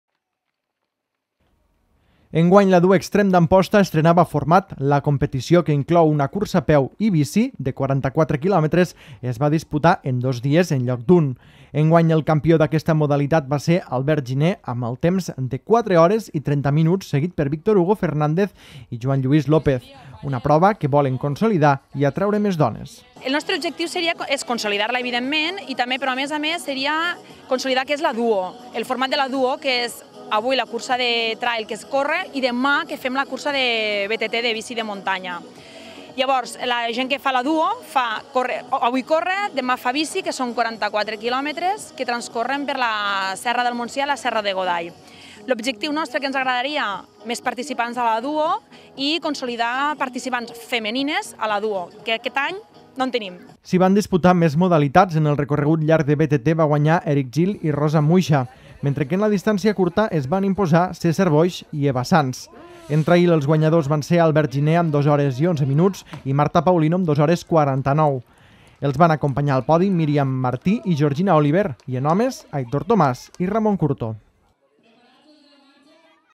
La Duextrem d'Amposta vol consolidar la prova combinada i atreure més participants
Enguany la Duextrem d’Amposta estrenava format. La competició que inclou cursa a peu i bici, de 44 quilòmetres es va disputar en dos dies, enlloc d’un. Enguany el campió d’aquesta modalitat va ser Albert Giné amb un temps de 4 hores i 30 minuts, seguit per Victor Hugo Fernández i Joan Lluís López. Una prova que volen consolidar i atreure més dones.
S’hi van disputar més modalitats. En el recorregut llarg de BBT va guanyar Èric Gil i Rosa Muixa, mentre que en la distància cursa es van imposar Cèsar Boix i Eva Sans. En trail, els guanyadors van ser Albert Giné, amb 2 hores i 11 minuts, i Marta Paulino amb 2 hores 49. Els van acompanyar al podi Miriam Martí i Georgina Olivier, i en homes Aitor Tomàs i Ramón Curto.